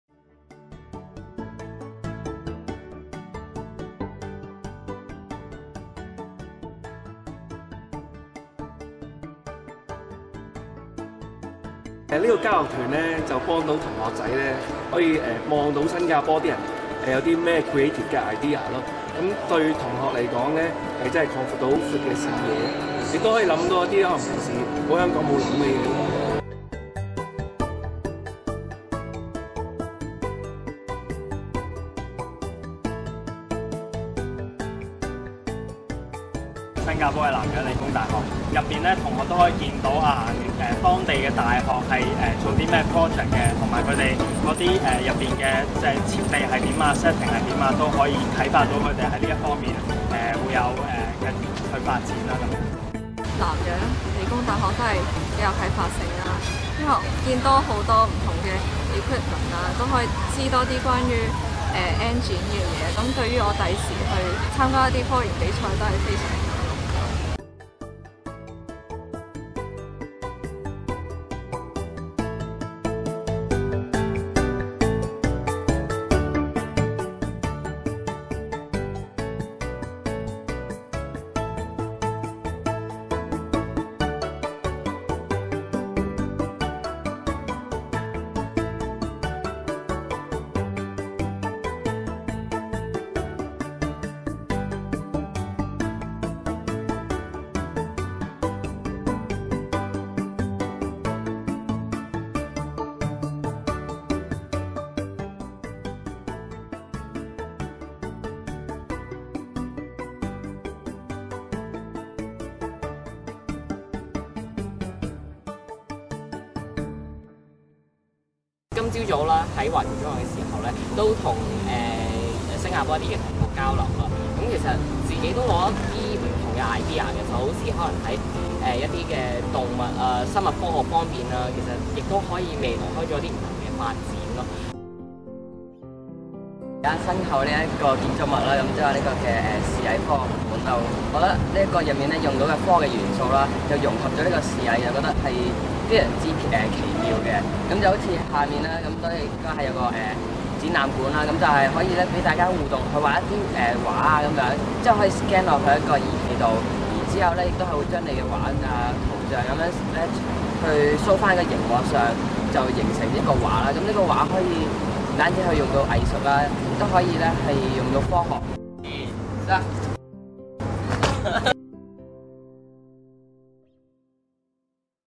2019 香港學生科學比賽新加坡交流團 2019 Singapore Exchange Tour
香港學生科學比賽的四支冠軍隊伍，包括20位學生和老師，獲贊助於2018年8月5至9日參與新加坡交流團。代表團參觀了南洋理工大學、華僑中學、藝術科學博物館、新加坡科學館、Canopy Park等，同學更於華僑中學與當地學生進行分享交流。透過交流，同學們不但擴闊了他們的國際視野，也更了解新加坡近年的科技發展。

From 5 to 9 August 2019, four winning teams, comprising 20 students and teachers, were sponsored to a Singapore exchange programme. They visited Nanyang Technological University, Hwa Chong Institution, ArtScience Museum, Singapore Science Museum and Canopy Walk etc. Students were also able to exchange ideas with local students in Hwa Chong. Through these exchanges, the students were able to broaden their global vision and understand the most updated scientific development in Singapore.